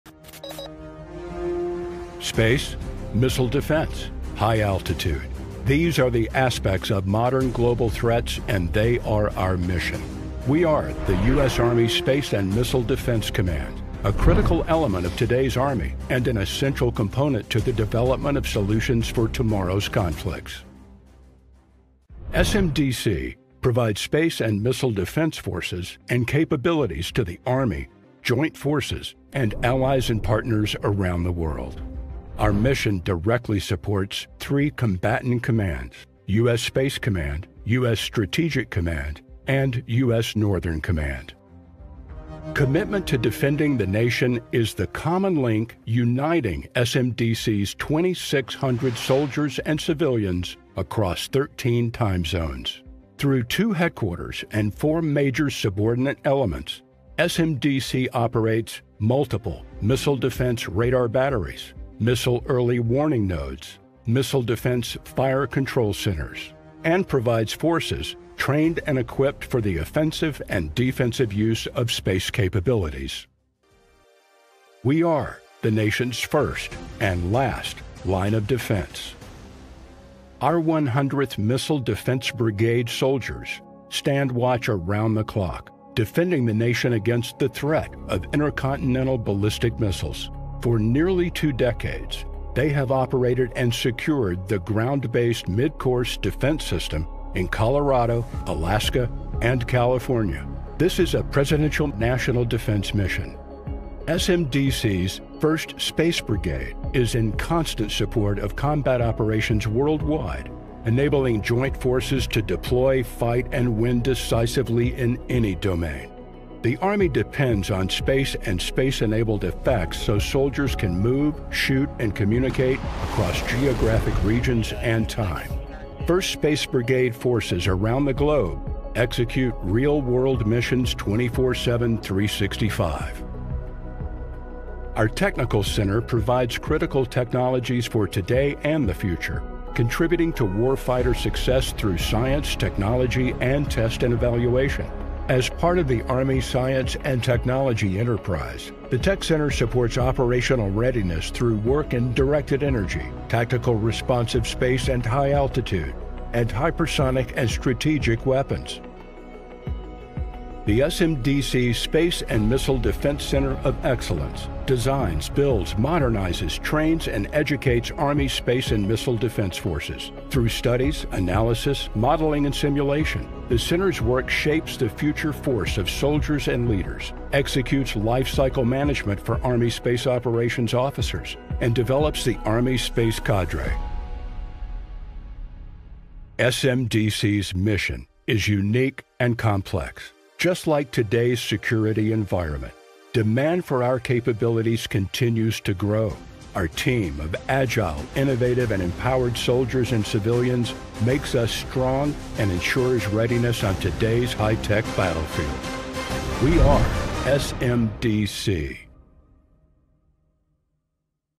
Space Missile Defense Command 2023
REDSTONE ARSENAL, AL, UNITED STATES
Video by Allen Meeks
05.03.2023
U.S. Army Space and Missile Defense Command
U.S. Army Space and Missile Defense Command, May 2023 command overview video.